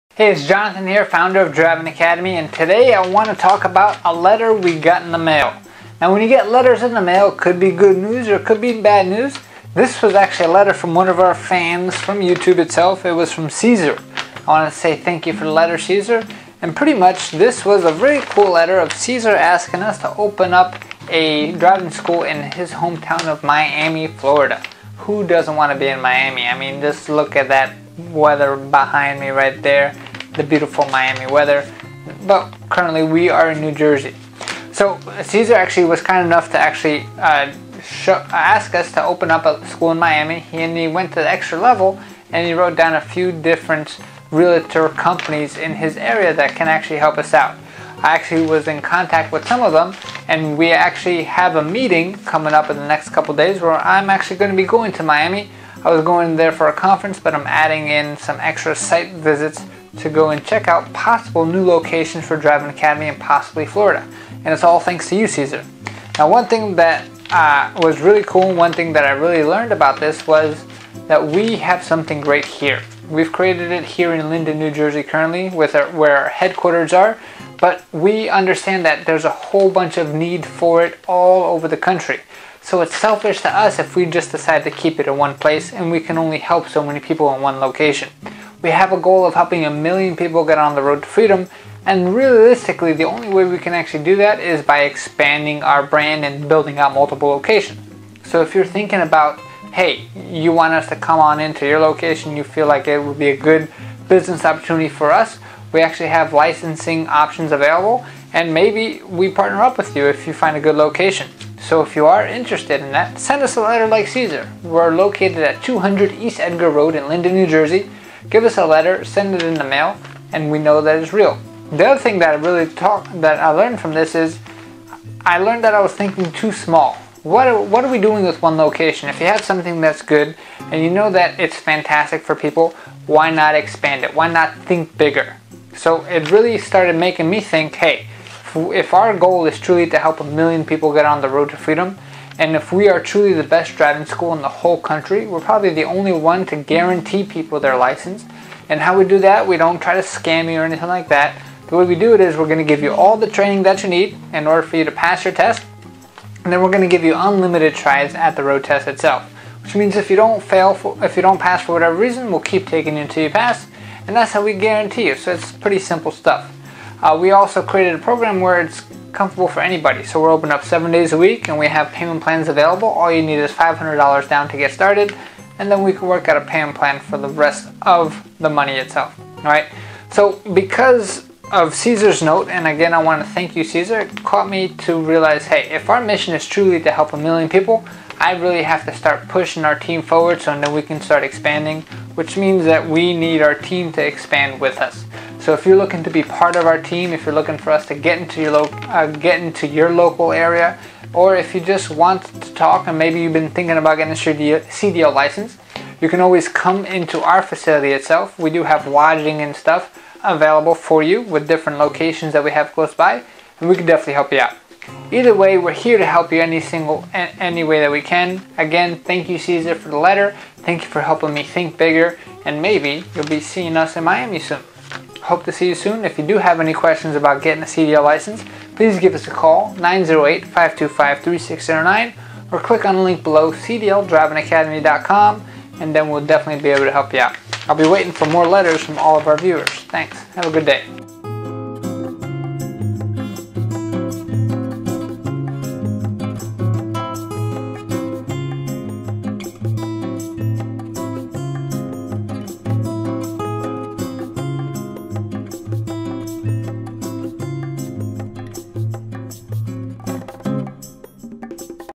Fan Letter! Send us Your Suggestions - CDL Driving Academy
Driving Academy

Jonathan from Driving Academy received a piece of mail from a fan in Miami, Florida.  Cesar, the sender, offered his suggestion on opening up a CDL driving school in Florida. Driving Academy is expanding, so if you need a CDL school in your area we are open to suggestions.

If you're looking to get your CDL and want to train with the BEST, give us a call TODAY!

200 East Edgar Road, Linden NJ